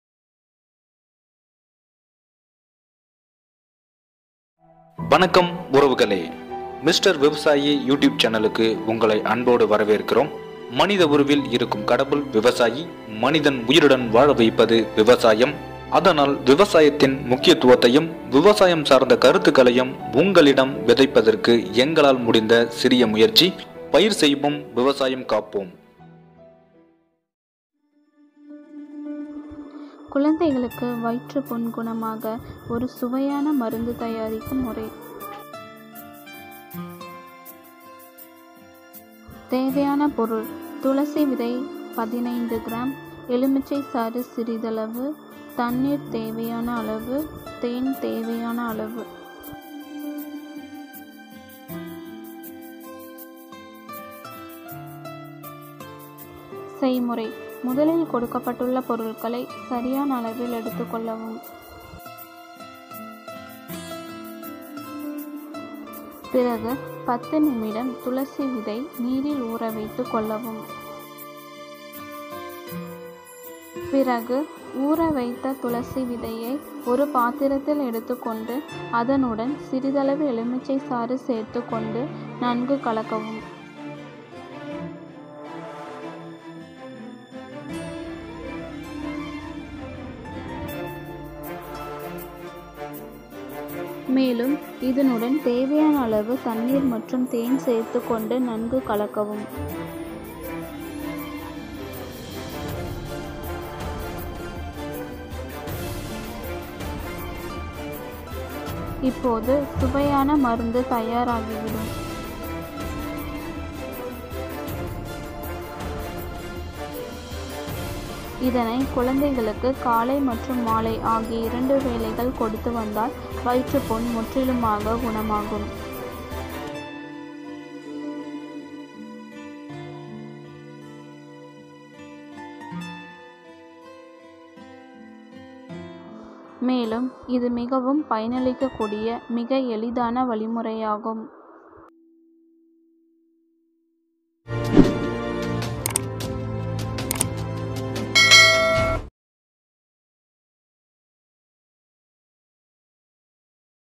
குழந்தைகளுக்கு வயிற்றுப்புண் குணமாக ஒரு சுவையான மருந்து தயாரிக்கும் முறை | MR. VIVASAYEE
குழந்தைகளுக்கு வயிற்றுப்புண் குணமாக ஒரு சுவையான மருந்து தயாரிக்கும் முறை